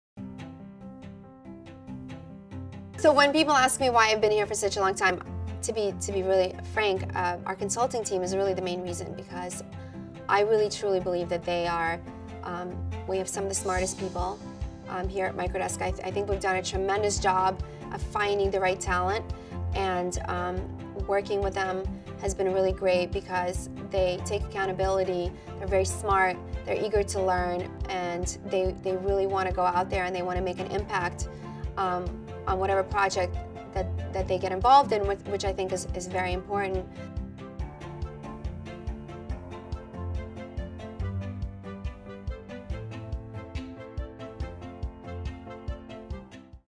Life@Microdesk: Finding Fulfillment Through Teamwork
Janet Pincus, Business Development Manager, shares her secret for staying with Microdesk for more than 10 years!